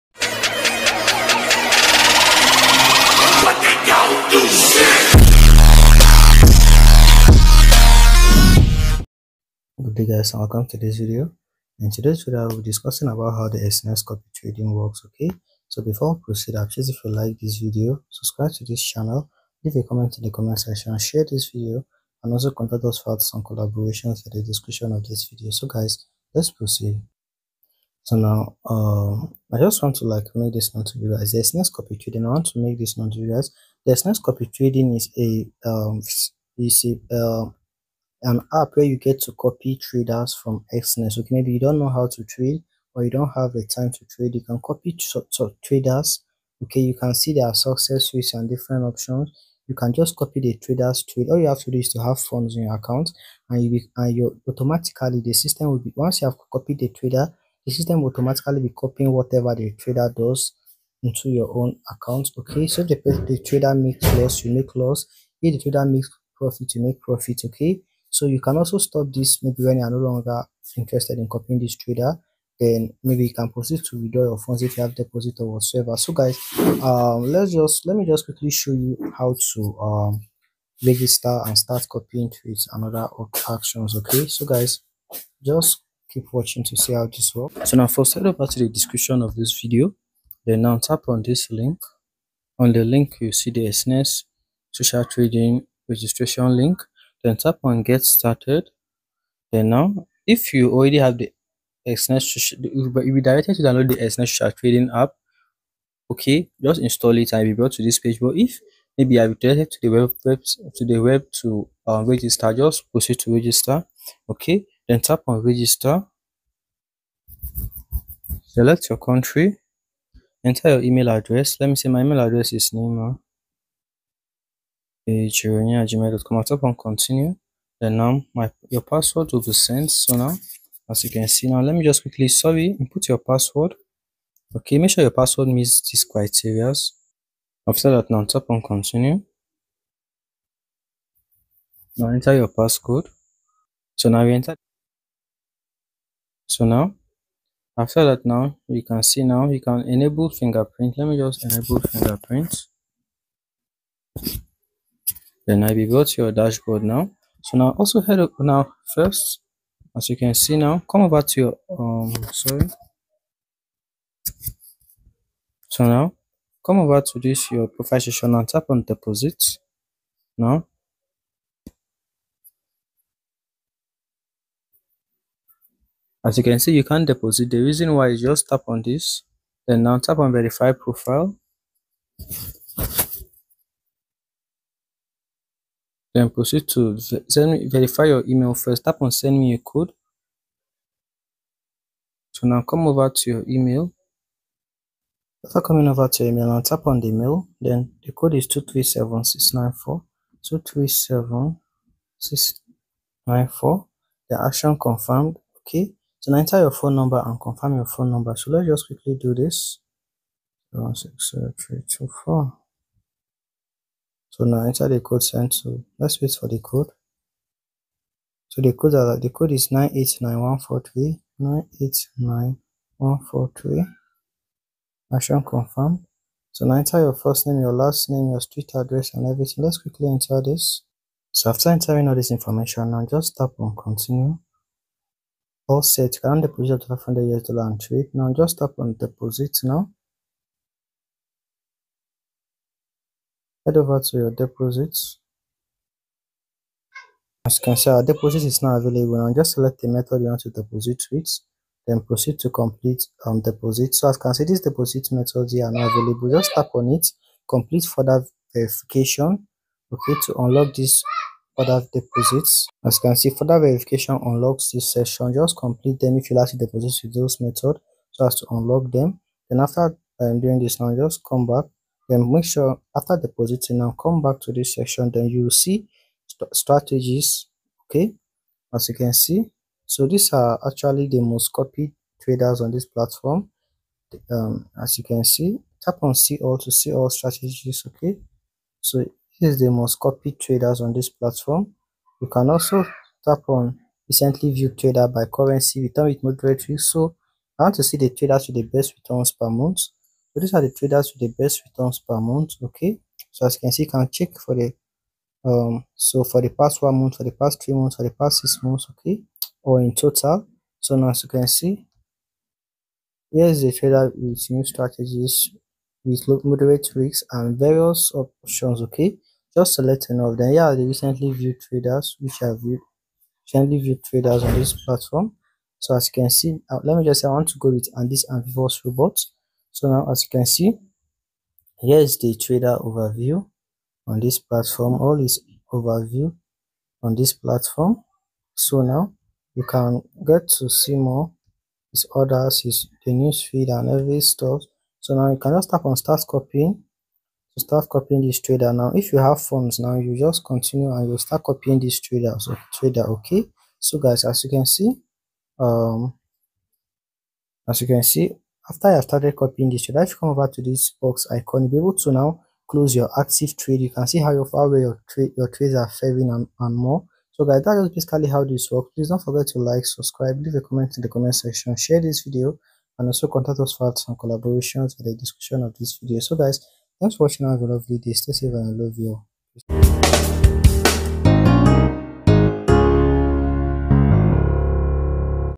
How Exness Copy Trading Works: Everything You Need To Know
Are you tired of trading alone and wanting to learn from the best? Exness Copy Trading is here to revolutionize your trading experience! In this video, we'll dive into the world of copy trading and explore how Exness can help you achieve your trading goals. From understanding how Exness Copy Trading works.

🎯 We'll cover everything you need to know to get started. Whether you're a beginner or an experienced trader, this video is perfect for anyone looking to take their trading to the next level. So, what are you waiting for? Watch now and discover the power of Exness Copy Trading!

?sub_confirmation=1

Twitter Handle

🔑 Related Keywords 🔑